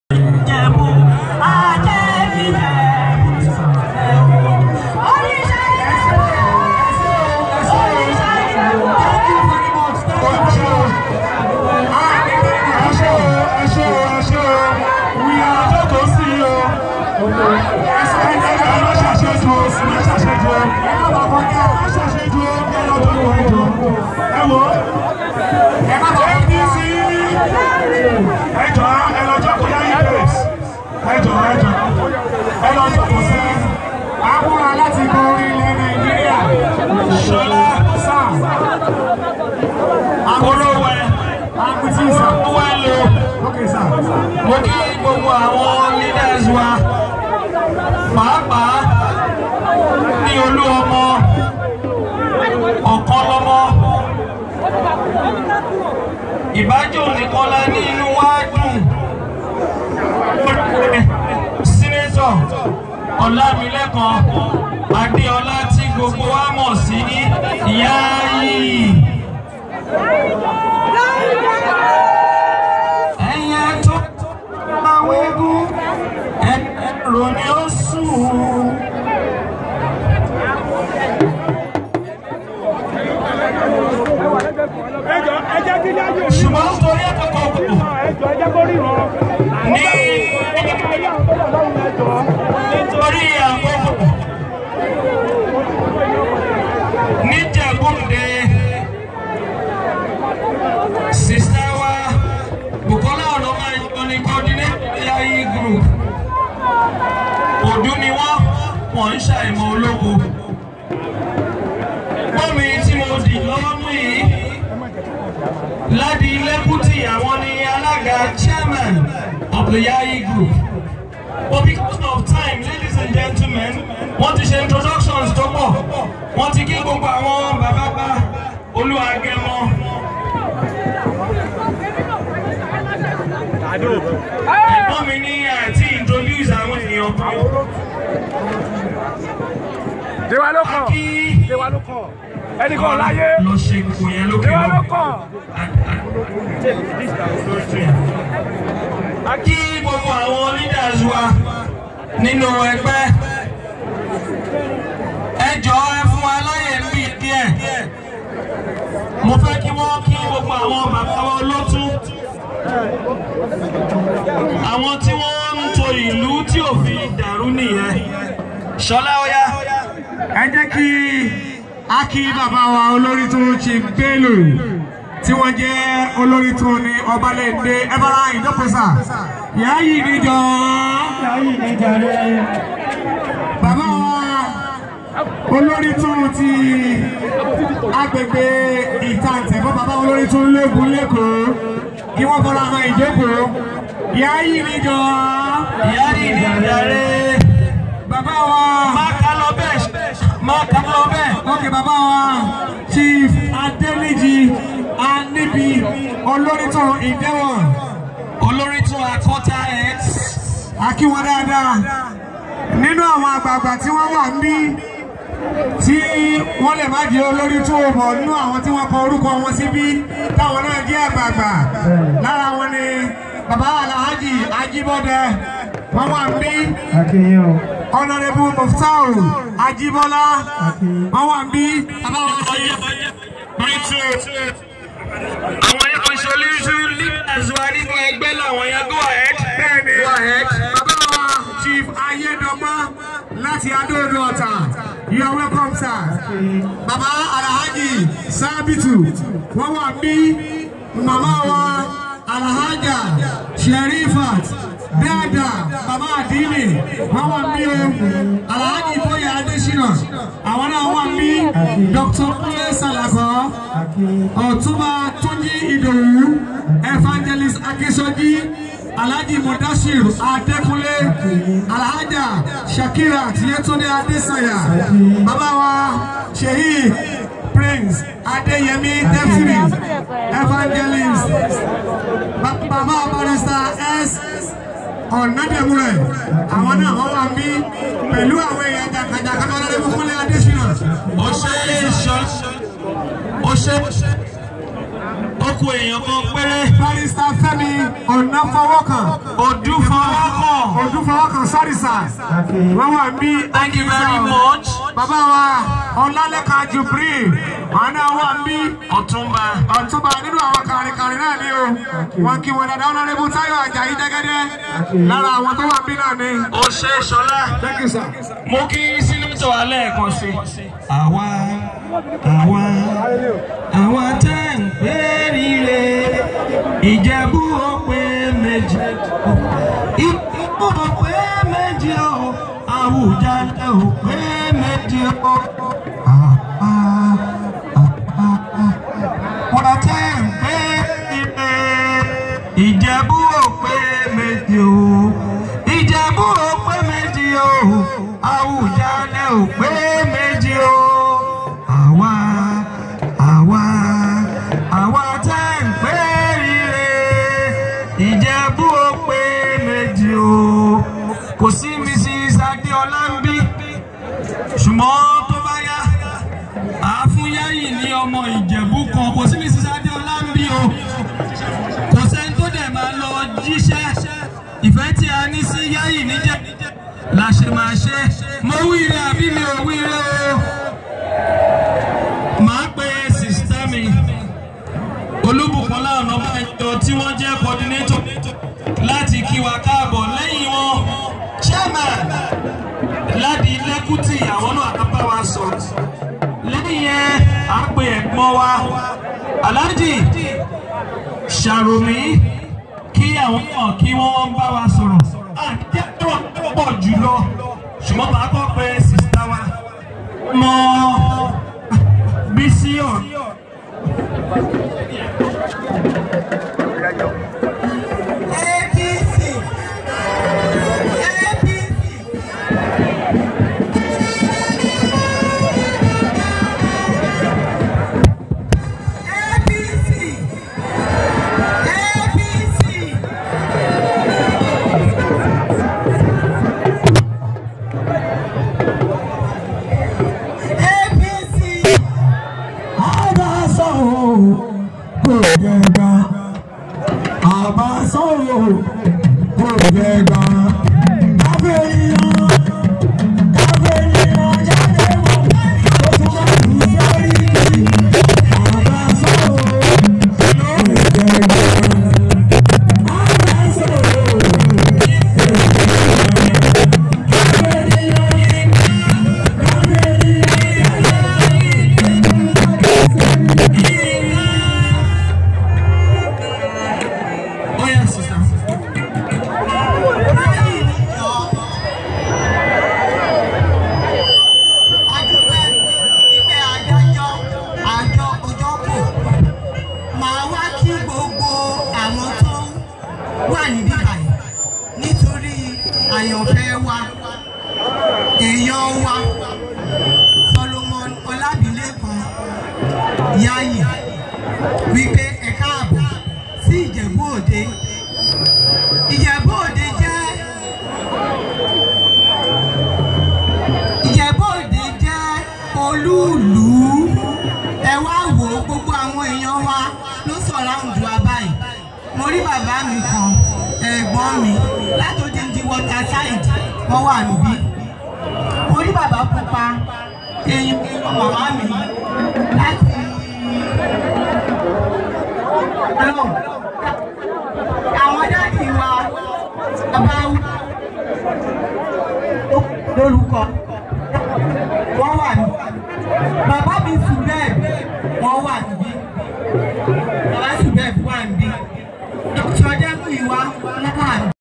SOLOMON ADEOLA YAYI CAMPAIGN WINS THE HEARTS OF IJEBU PEOPLE
SOLOMON ADEOLA YAYI BEGINS CAMPAIGN AT IJEBUODE ON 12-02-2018